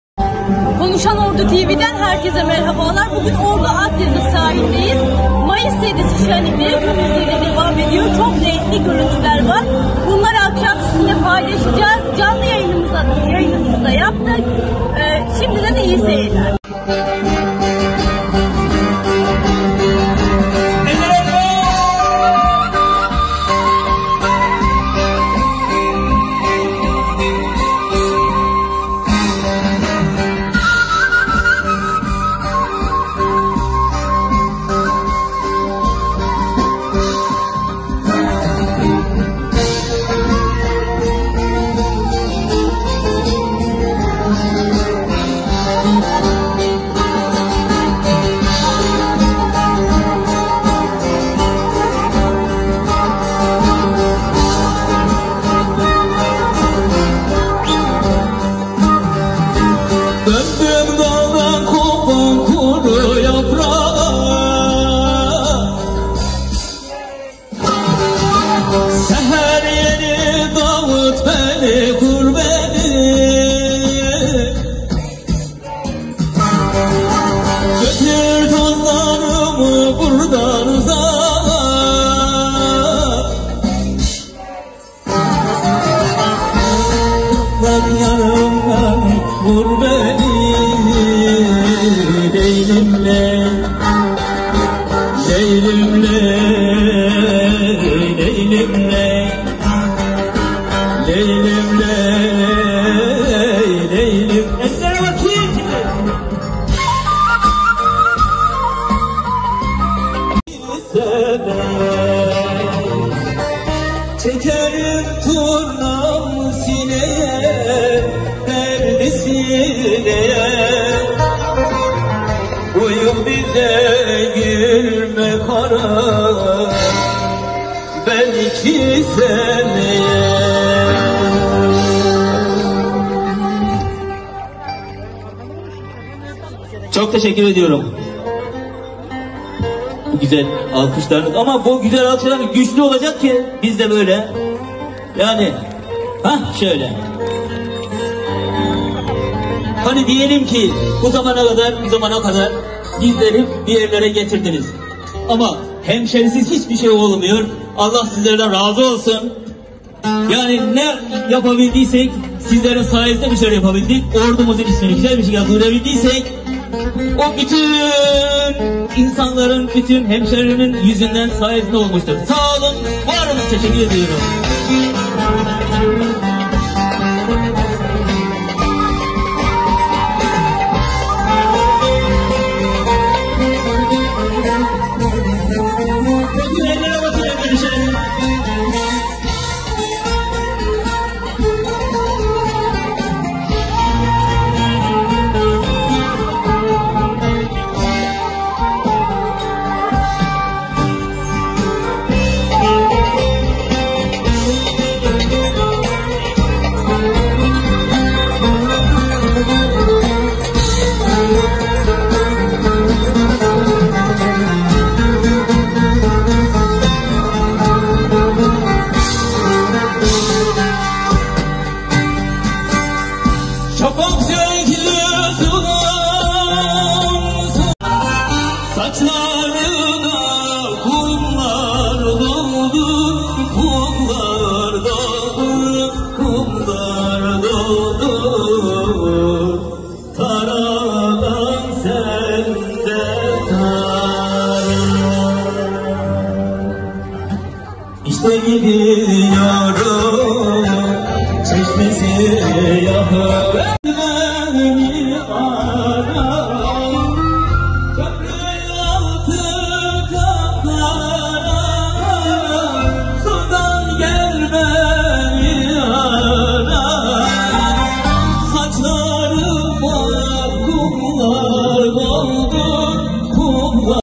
ORDU'DA ŞENLİK VAR
Altınordu Belediyesi Geleneksel Mayıs 7'si Şenlikleri Kapsamında Etkinlikler Düzenliyor. Konuşan Ordu Ekibi Sahada !